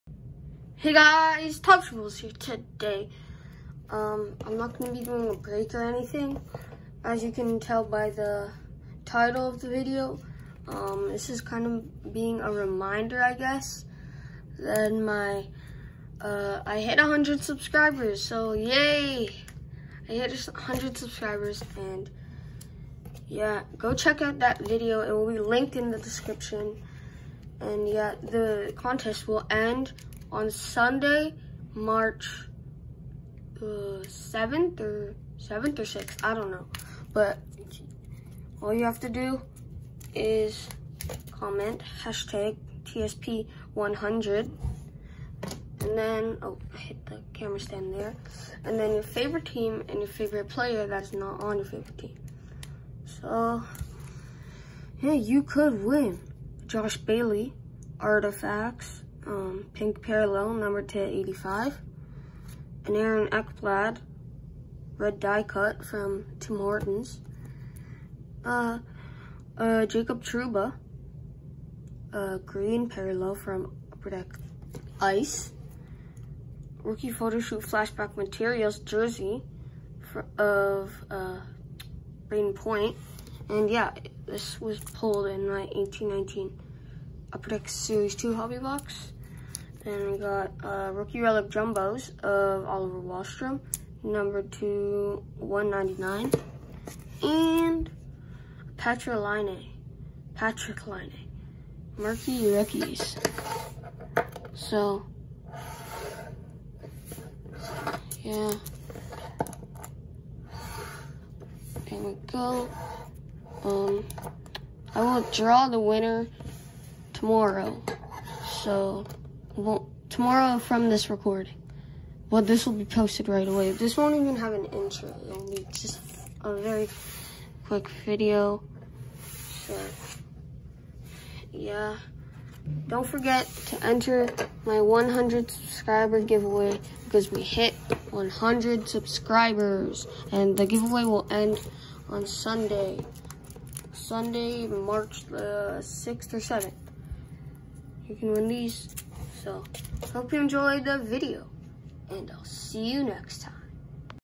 We Hit 💯!!! 🎉🎉 - 100 Subscriber Giveaway Reminder
My 100 Subscriber Giveaway will end on Sunday, March 7th 2021.

Don’t forget to enter!